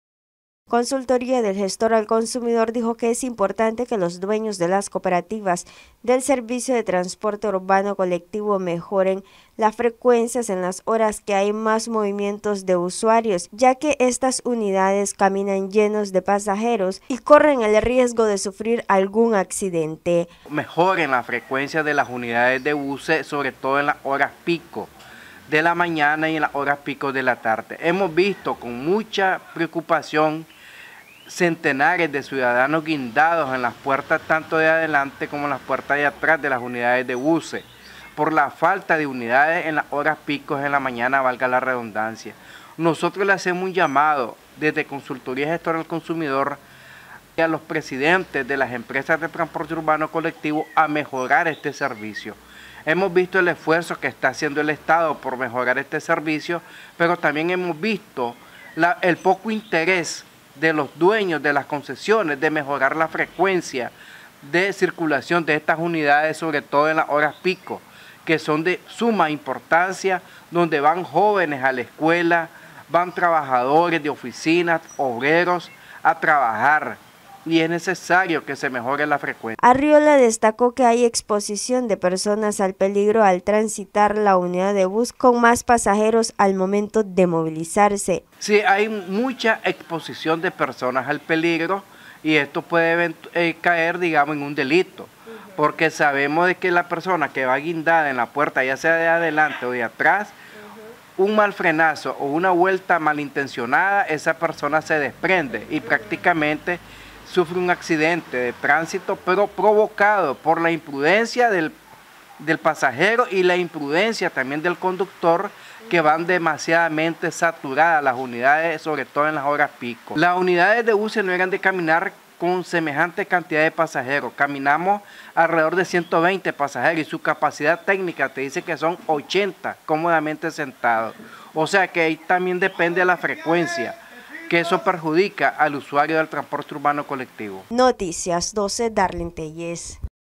Noticias12Nicaragua Urge mejorar la frecuencia de las unidades de buses en Managua
Urge mejorar la frecuencia de las unidades de buses en Managua asegura defensor de los consumidores
Defensor de los Consumidores, dijo que es importante que se
mejore la frecuencia de las unidades de buses en Managua y
evitar que haya algún accidente por exceso de pasajeros